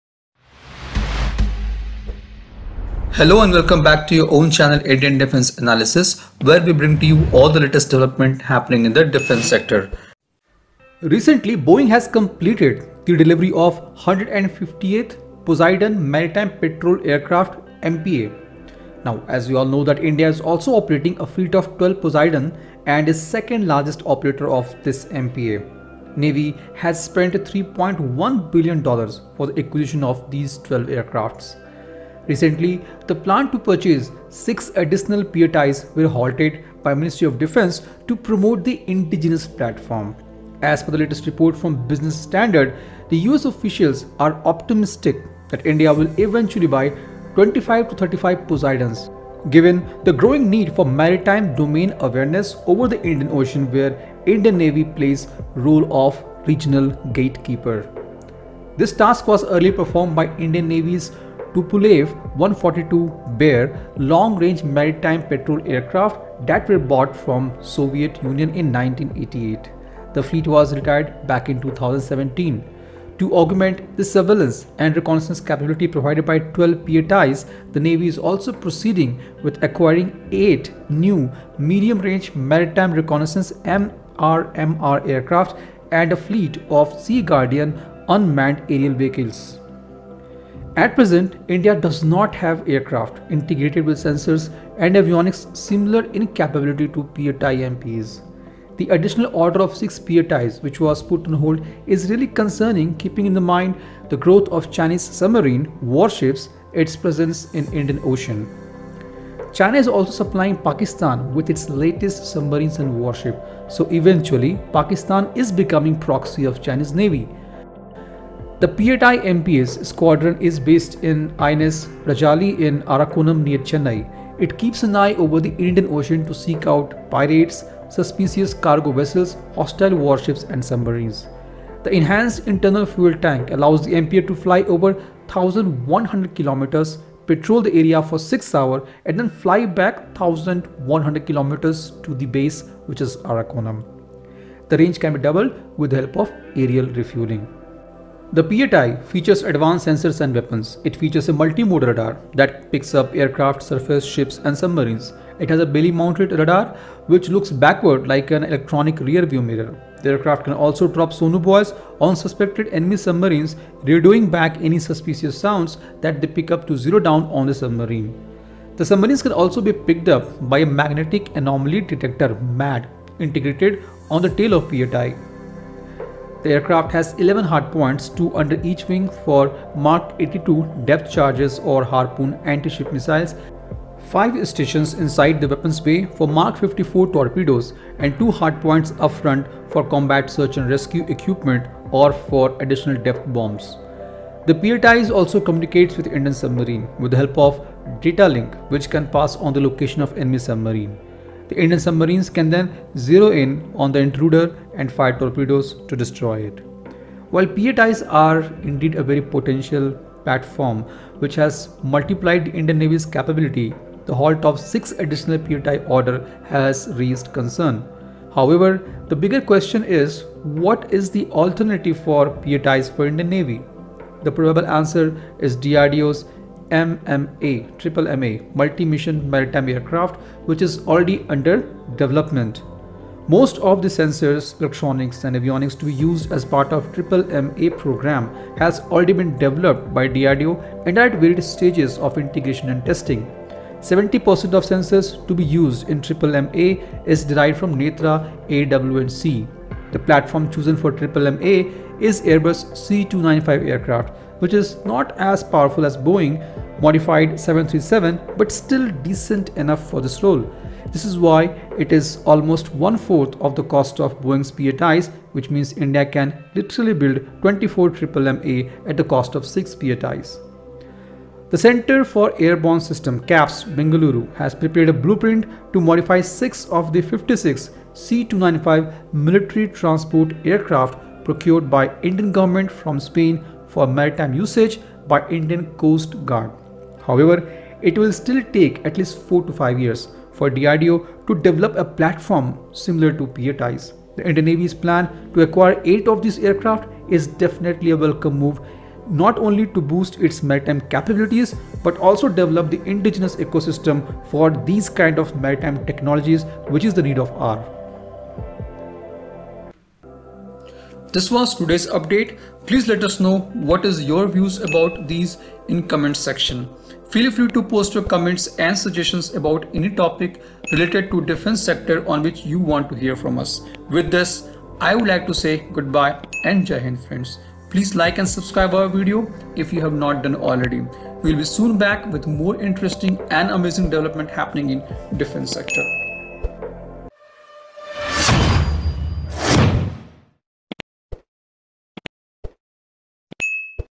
Indian navy proceeding with acquiring 8 MMRs, 10 Sea Guardian UAVs & P-8Is
8 MMRs +10 Sea Guardian UAVs + 25-35 P-8I MPAs for Indian Navy

Recently Boeing has completed the delivery of 150th Poseidon maritime petrol aircraft. As we all know that India is also operating a fleet of 12 Poseidons and is 2nd largest operator of the MPA. Navy has spent $3.1 Billion for the acquisition of these 12 aircrafts. Recently the plan to purchase 6 additional P-8Is were halted by MoD to promote the indigenous platform. As per the latest report from Business Standard, US officials are optimistic that India will eventually buy 25-35 Poseidons, given the growing need for “maritime domain awareness” over the Indian Ocean, where the Indian Navy plays the role of regional gatekeeper. This task was earlier performed by the Indian Navy’s Tupolev-142 Bear long-range maritime patrol aircraft that were bought from the Soviet Union in 1988. The fleet was retired in 2017. To augment the surveillance and reconnaissance capability provided by twelve P8-Is, the navy is also proceeding with acquiring eight new Medium Range Maritime Reconnaissance (MRMR) aircraft, and a fleet of Sea Guardian unmanned aerial vehicles (UAVs).

Need of MPA for Navy

At present, India doesn’t have the aircraft integrated with sensors and avionics similar to P-8I MPAs. The additional order of 6 P-8Is which was put on hold is really concerning keeping in mind the growing Chinese submarine, warships and its presence in Indian Ocean. China is also supplying Pakistan with its latest submarines and warships. So eventually Pakistan is becoming the proxy of Chinese Navy.

The P-8I MPAs squadron is based in INS Rajali, at Arakonam, near Chennai. It keeps an eye over the Indian Ocean to seek out pirates, suspicious cargo vessels, hostile warships and submarines. The enhanced internal fuel tanks allows the MPA to fly over 1,100 kilometers, patrol the area for six hours, and then fly back 1,100 kilometres to Arakonam. The range can be doubled with the help of aerial refuelling.

P-8I features & capabilities

The P8-I features advanced sensors and weapon. It features a multi-mode radar that picks up aircraft, surface ships and submarines. It has a belly-mounted radar which looks backward, like an electronic rear-view-mirror. The aircraft can also drop sonobuoys on suspected enemy submarines, radioing back any suspicious sounds that they pick up to zero down on the submarine. Submarines can also be picked up by a magnetic anomaly detector (MAD) integrated on the tail of P-8I.  It has 11 “hard points”: two under each wing for Mark 82 depth charges or Harpoon anti-ship missiles; five stations inside the weapons bay for Mark 54 torpedoes and two hard points up front for combat search and rescue (SAR) equipment or for additional depth bombs.

The P8-Is also communicates with Indian Submarine with the help of datalink which can pass on the location of the enemy submarine. The Indian submarines can then zero-in on the intruder and fire torpedoes to destroy it.

DRDO’s MMMA

While P-8Is are indeed a very potent platform which has multiplied the Indian Navy’s capability, the halt of 6 additional P-8Is has raised concerned. However, the bigger question is what the alternative of P-8Is for Indian Navy. The probable answer is DRDO’s Multi-Mission Maritime Aircraft (MMMA) which is already under development. Most of the sensors/electronics and avionics to be used as a part of MMMA program has already been developed by DRDO and are at various stages of integrations and testing. 70% of sensors to be used in MMMA is derived from Netra AEW&C. The platform chosen for MMMA is Airbus C-295 aircraft which is not as powerful as Boeing’s modified 737 but still decent enough for this role. This is why it is almost one fourth of the cost of Boeing’s P-8Is which means India can build 24 MMMA at the cost of 6 P-8Is. The Centre for Airborne Systems (CABS), Bengaluru, has prepared a blueprint to modify six of the 56 C-295 military transport aircraft procured by the Indian government from Spain for maritime usage by the Indian Coast Guard.